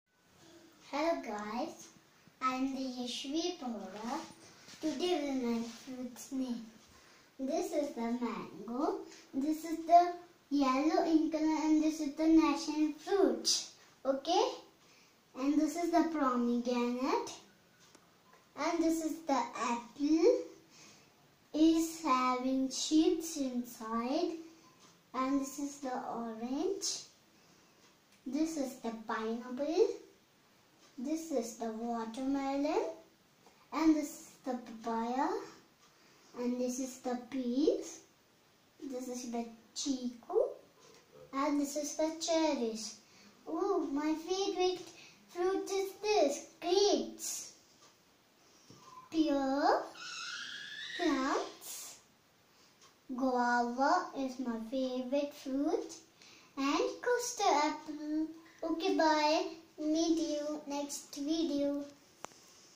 Fruits name by Yashvi Bohra
Yashvi Bohra learning fruits name apple, orange, banana, grape, grapes, kiwi, kiwi fruit, kiwifruit, pineapple, lemon, watermelon, pear etc.